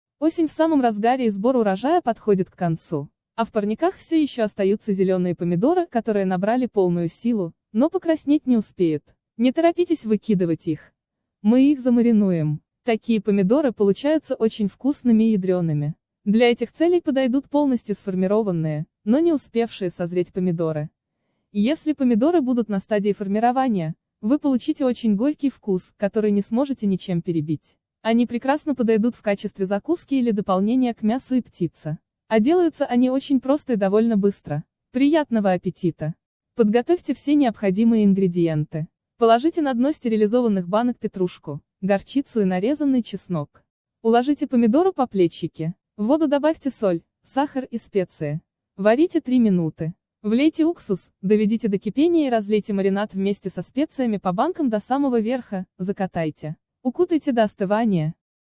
Маринованные зеленые помидорки. Условиях пошаговый рецепт
Осень в самом разгаре и сбор урожая подходит к концу. А в парниках все еще остаются зеленые помидоры, которые набрали полную силу, но покраснеть не успеют. Не торопитесь выкидывать их. Мы их замаринуем!    Такие помидоры получаются очень вкусными и ядреными. Для этих целей подойдут полностью сформированные, но не успевшие созреть помидоры. Если помидоры будут на стадии формирования, вы получите очень горький вкус, который не сможете ни чем перебить. Они прекрасно подойдут в качестве закуски или дополнения к мясу и птице. А делаются они очень просто и довольно быстро. Приятного аппетита!

Ингредиенты:
Помидор — 1.3 Килограмма (зеленые),
Соль — 1 Ст. ложка,
Сахар — 4 Ст. ложки,
Уксус 9% — 100 Миллилитров,
Гвоздика — 4 Штуки,
Перец душистый — 12 Штук,
Лавровый лист — 2 Штуки,
Перец черный — 15 Штук,
Петрушка — 30 Грамм,
Горчица листовая — 20 Грамм,